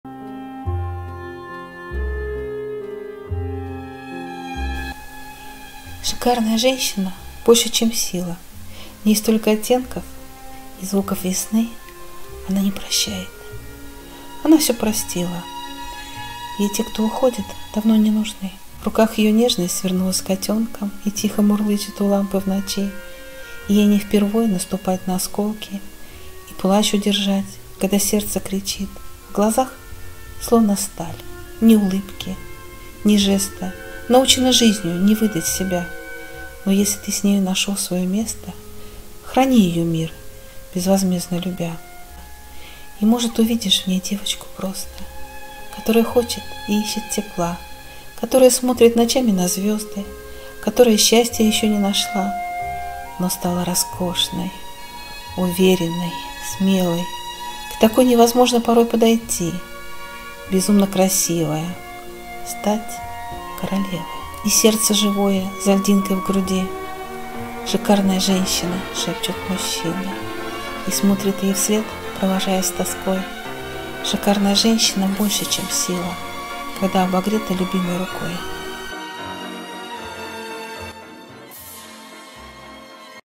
Шикарная женщина. Автор Татьяна Лопатина. Стихи о жизни.
Современная поэзия.